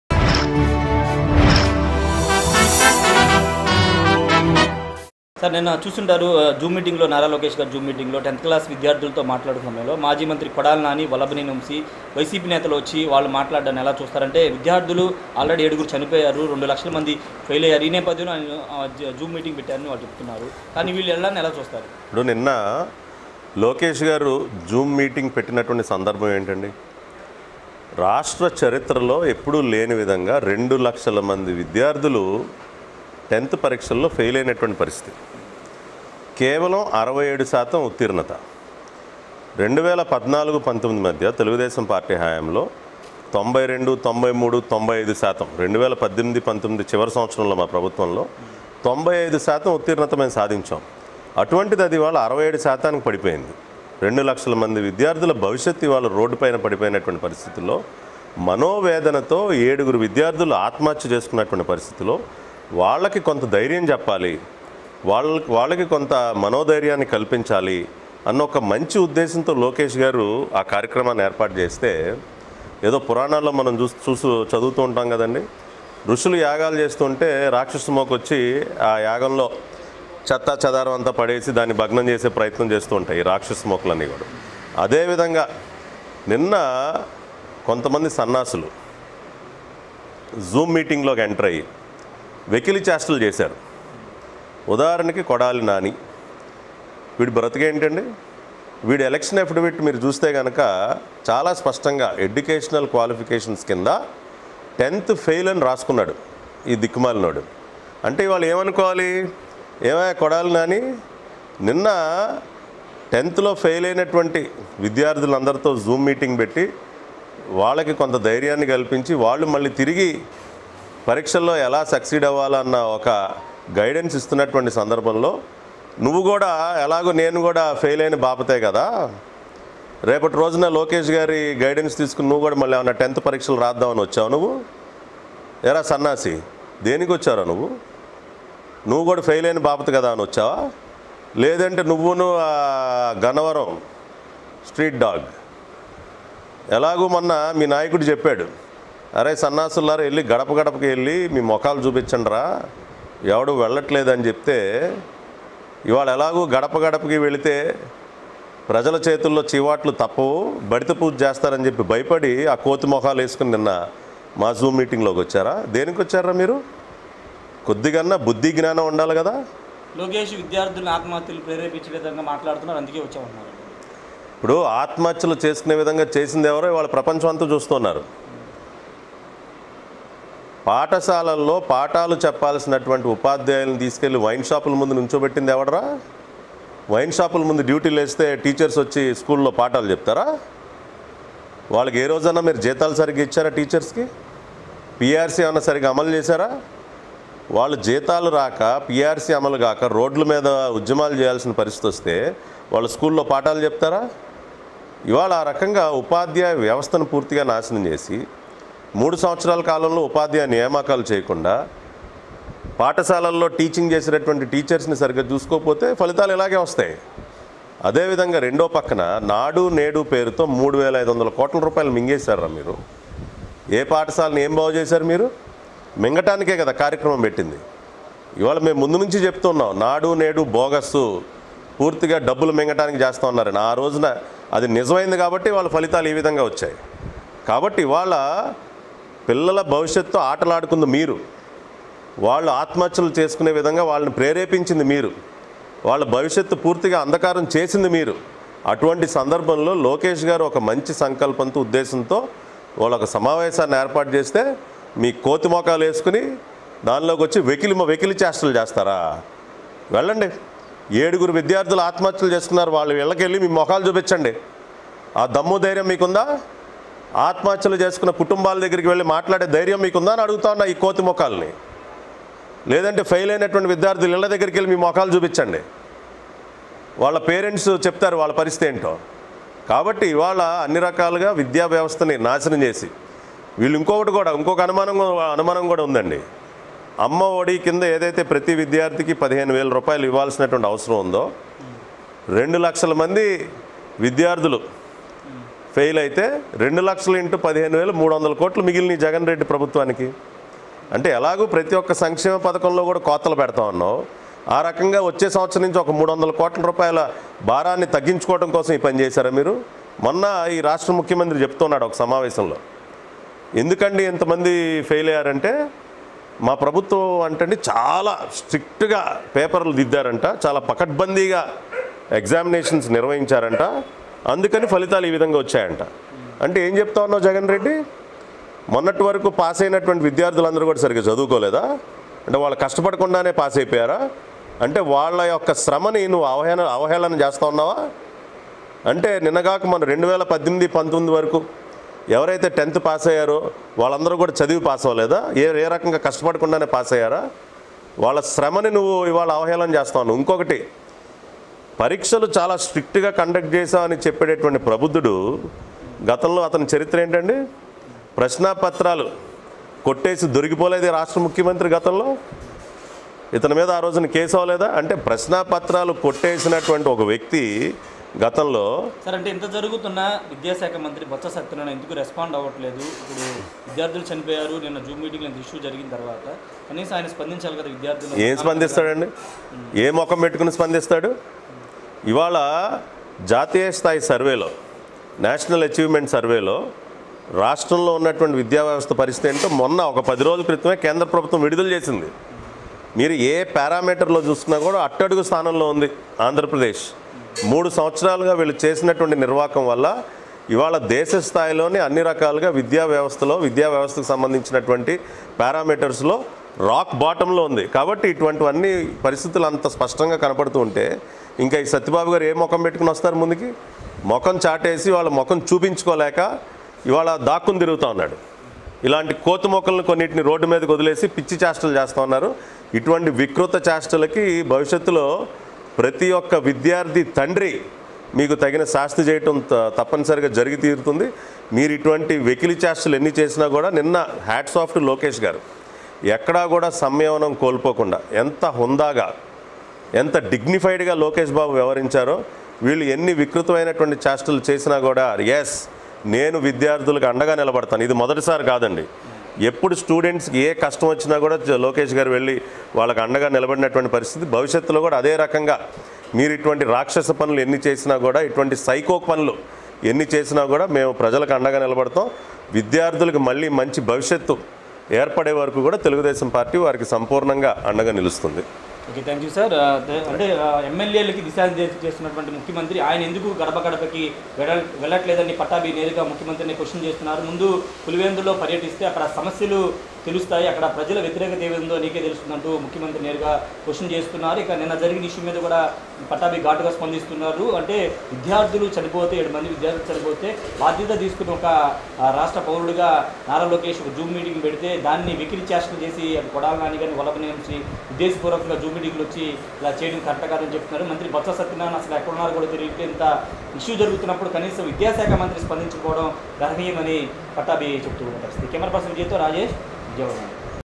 10వ తరగతి కూడా పాస్ కానీ కొడాలి నాని లోకేష్ పై విమర్శలు చేస్తునాడు | Kodali Nani | Maarpu TV
10వ తరగతి కూడా పాస్ కానీ కొడాలి నాని లోకేష్ పై విమర్శలు చేస్తునాడు | Kodali Nani | Maarpu TV